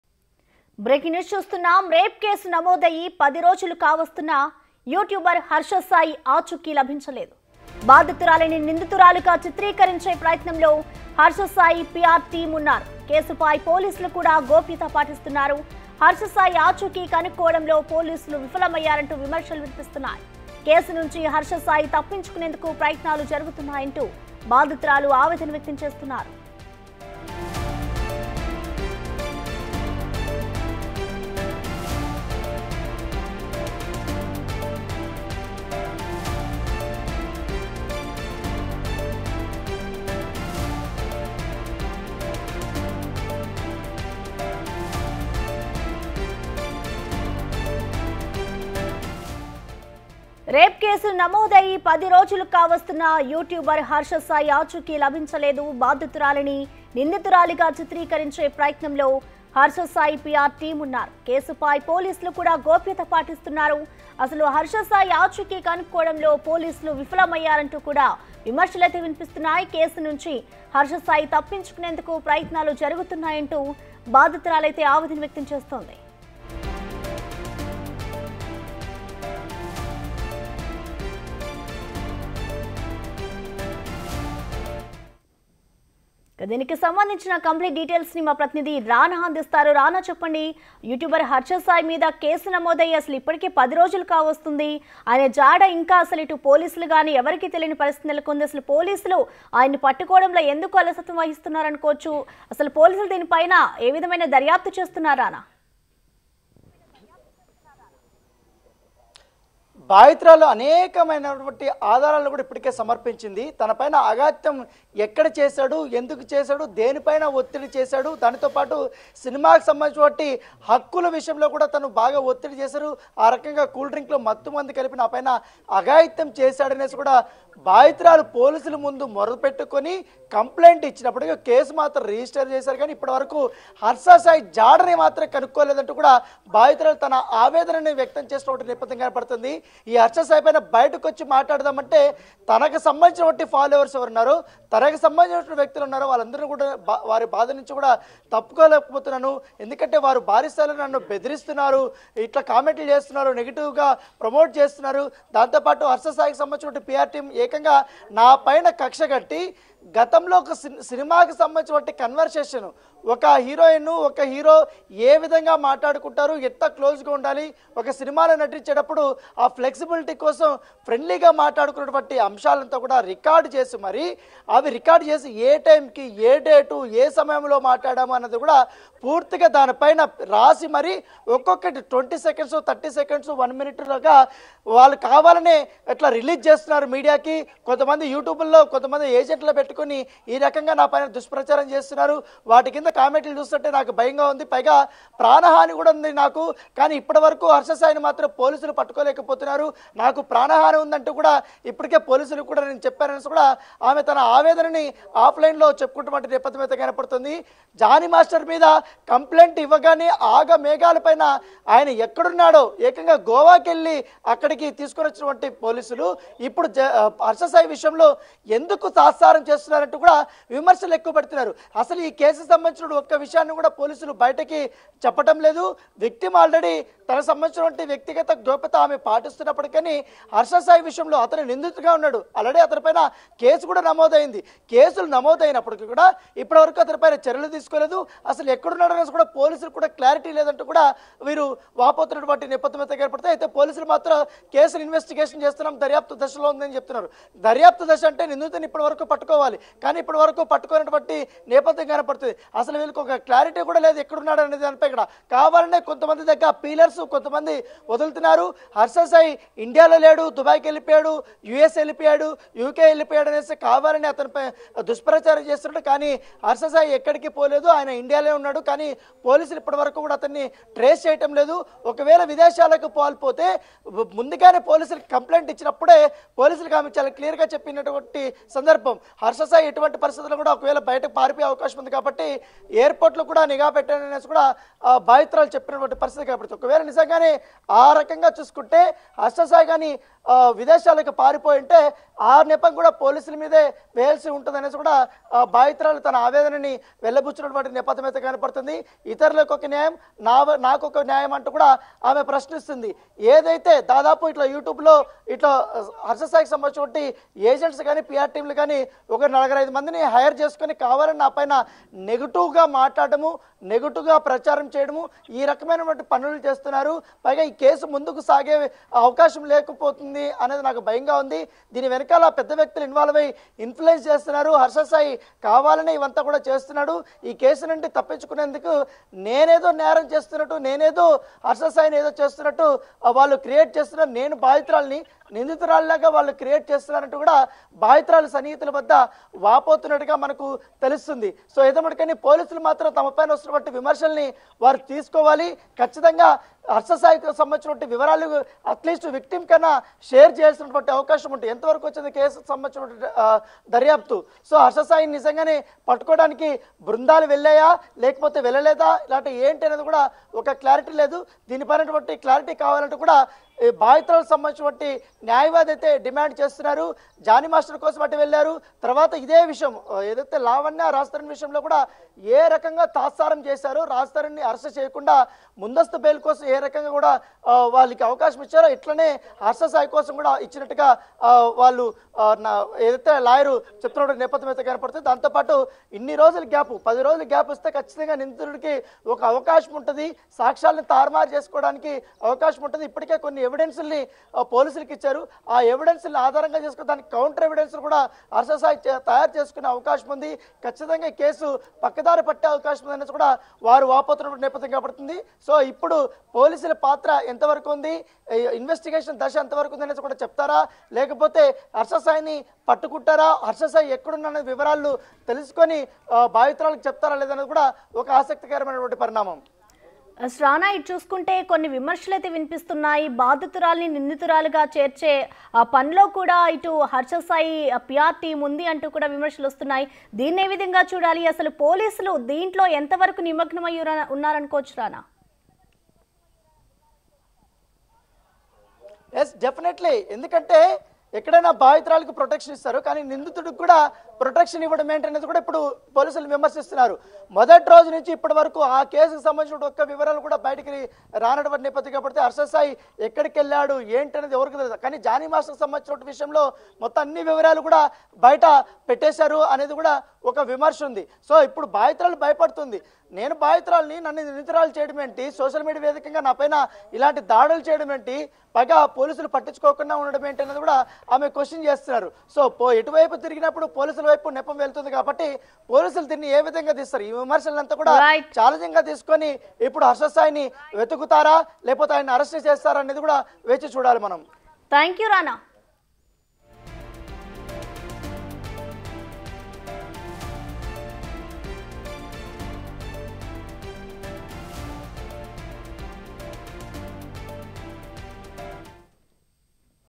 హర్ష సాయి అరెస్ట్ ఆలస్యం.. వాళ్ళ కుట్ర? | Youtuber Harsha Sai Case Updates | BIG TV
హర్ష సాయి అరెస్ట్ ఆలస్యంపై వాళ్ళ కుట్ర? |  Youtuber Harsha Sai Case Updates | BIG TV